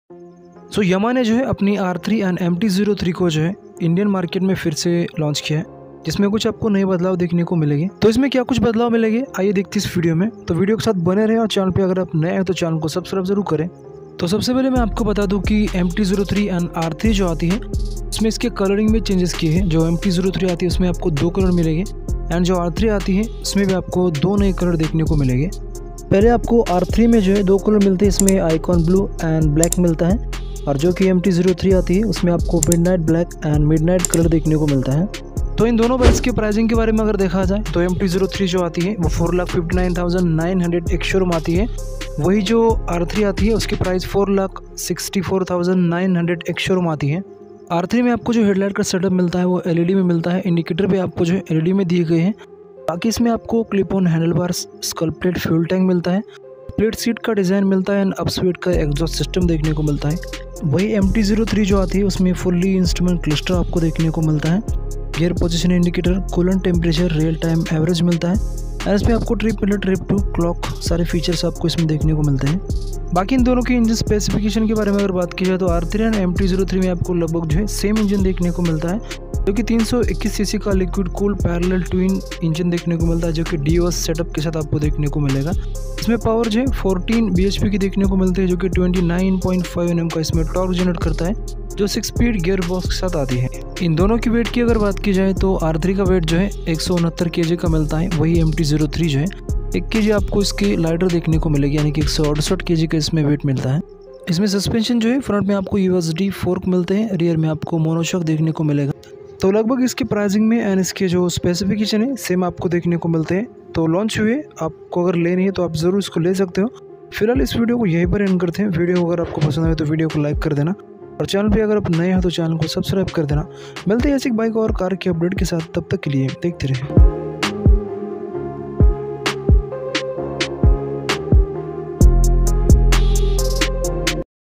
2024 Yamaha MT 03 And R 3 Launched in India 💥start From RS 4.6 Lakh || all details
2024 Yamaha MT 03 And R 3 Launched in India 💥start From RS 4.6 Lakh || all details